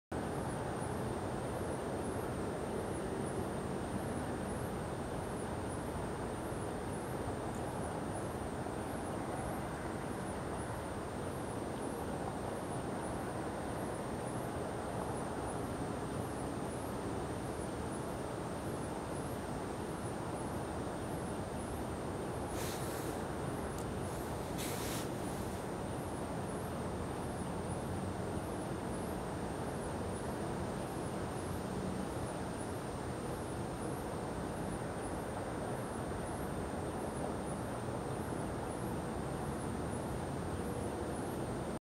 2015.05.21 - Saturn from my backyard
Horrible seeing conditions, but it's so cool to finally see the rings with my own eyes!

Orion 8" f/3.9 Newtonian Astrograph
TeleVue 2x 2" PowerMate
Canon 60D (640x480 video crop mode)